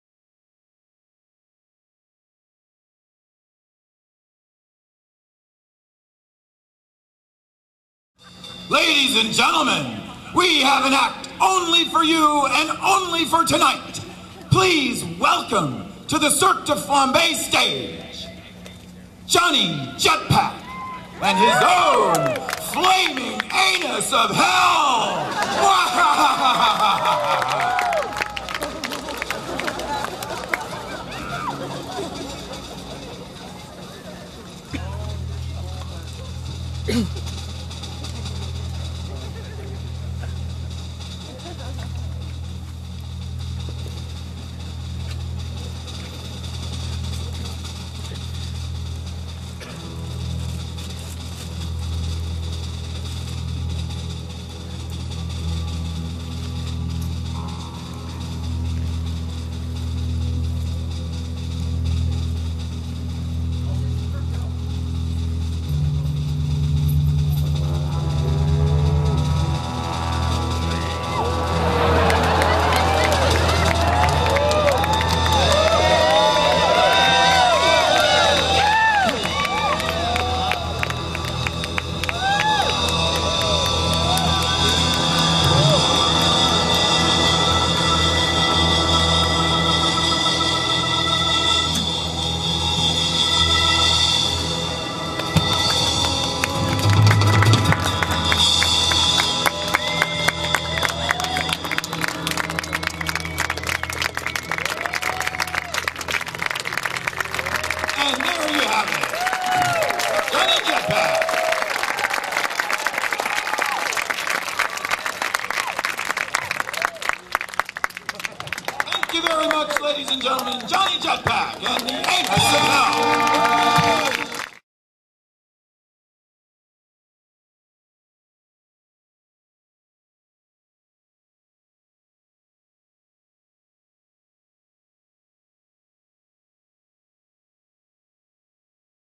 Johnny Jetpack's Flaming Anus of Hell
Johnny Jetpack performs his famous Flaming Anus of Hell act during the Cirque de Flambe's UH! OH! PLAYING WITH FIRE show.